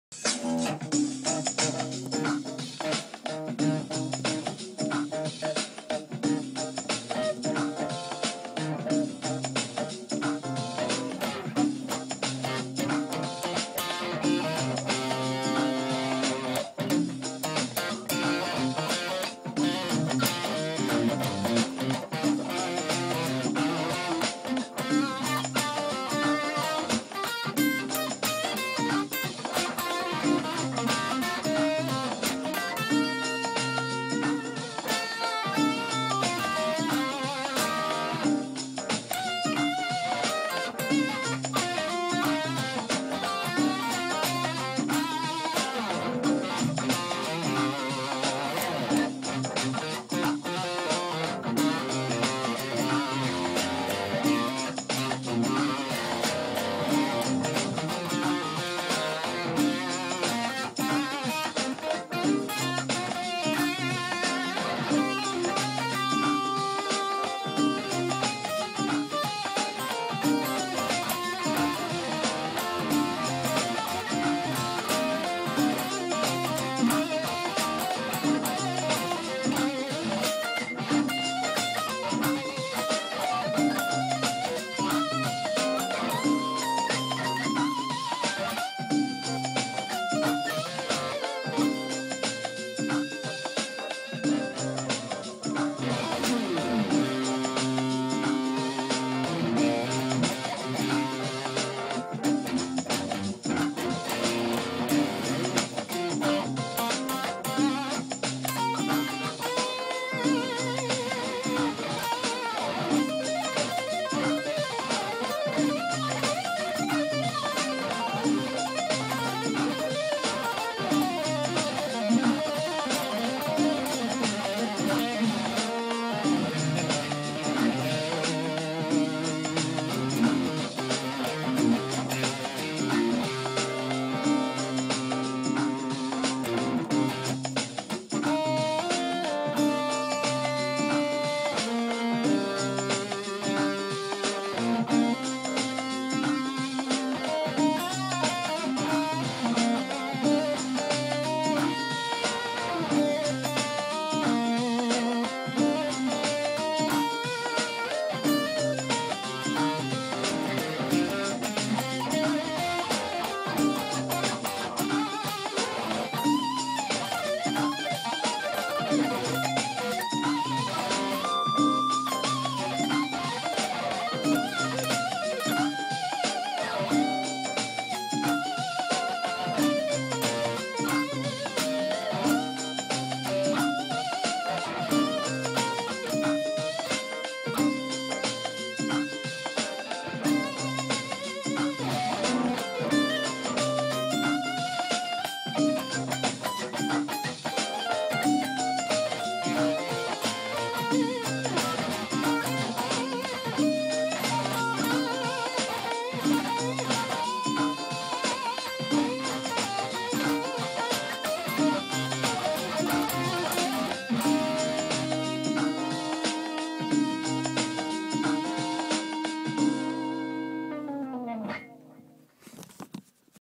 "DRUM MACHINE SEX GUITAR SHRED(guitar improv over groovin' backing track)
electric guitar improvisation over a backing track in c minor- THIS IS NOT MY BACKING TRACK- Just something I clicked on and improvised over off the top of my head- thanks to the owner of the track- please subscribe to my channnel at: jamespatricksVIDZ or friend me on Facebook: JamesPatrickCunningham peace out! :-) and keep on rockin!